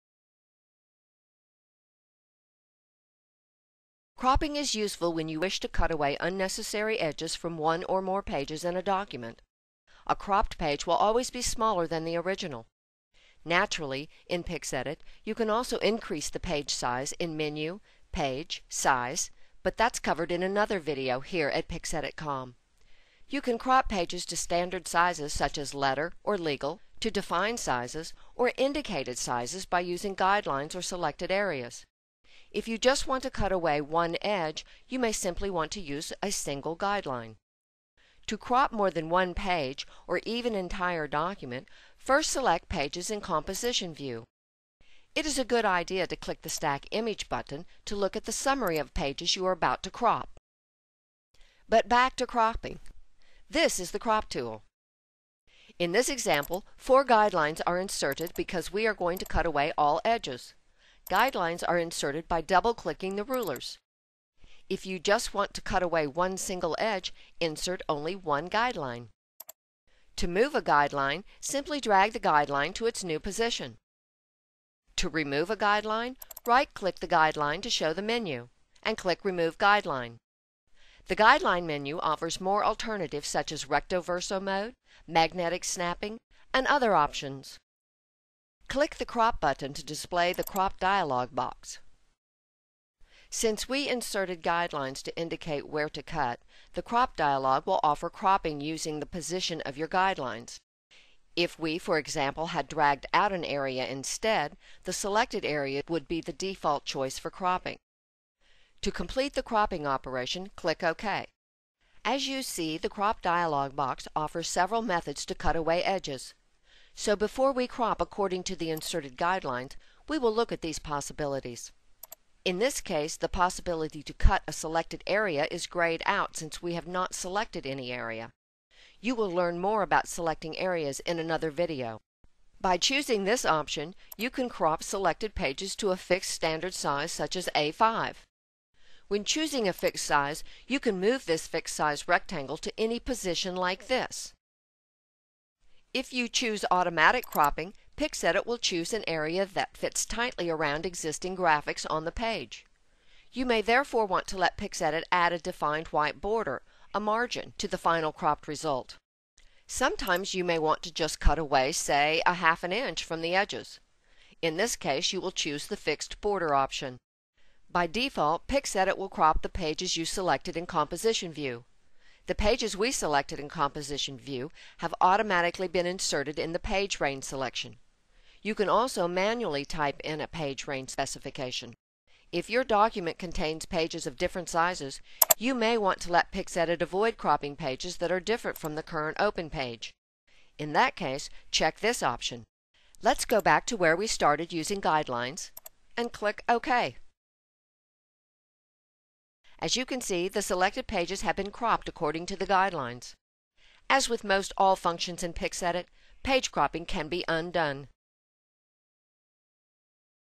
25 Cropping scans, images and pages in PixEdit Document Scanner Software.
Here you will learn several different ways to crop pages, in scanned documents and drawings, removing unwanted raster data from PDF, TIFF and JPEG images.  Image croping can be done individaully or run as a huge batch.  for further information.